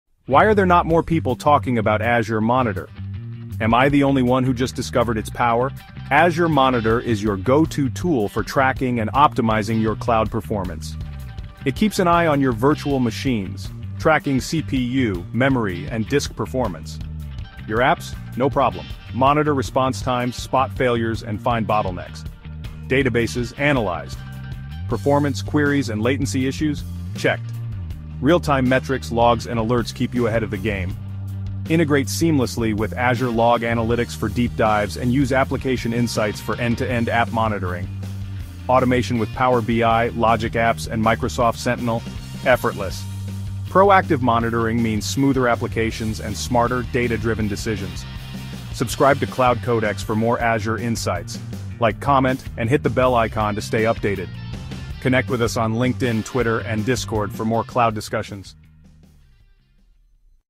📊 Azure Monitor – Real-Time Cloud Monitoring & Insights | Explained! 🚀
Monitoring cloud resources is essential for performance, security, and reliability. In this video, we break down Azure Monitor, Microsoft’s real-time cloud monitoring tool, which helps track and analyze Azure resources, applications, and infrastructure.

🔹 What You’ll Learn in This Video:
✅ What is Azure Monitor? A powerful tool for tracking cloud performance.
✅ How Azure Monitor collects & analyzes data from:
    📊 Virtual Machines (VMs) – Monitor CPU, memory, and storage usage.
    📦 Applications – Gain insights into response times and failures.
    📂 Databases – Analyze query performance and latency issues.

✅ Real-time metrics, logs & alerts 📈 – Get instant notifications about issues.
✅ Azure Log Analytics integration 🔍 – Advanced troubleshooting & diagnostics.
✅ Application Insights 📊 – Full-stack monitoring for web and cloud applications.
✅ Seamless automation with Power BI, Logic Apps & Microsoft Sentinel ⚙️ – Automate issue detection and response.

🎥 This video includes Azure service icons for each feature to help you understand visually! 🖼️

With Azure Monitor, businesses can proactively detect performance issues, ensure high availability, and make data-driven decisions for their cloud infrastructure.

📩 Let’s connect! Join us on LinkedIn, Twitter & Discord for more cloud computing discussions.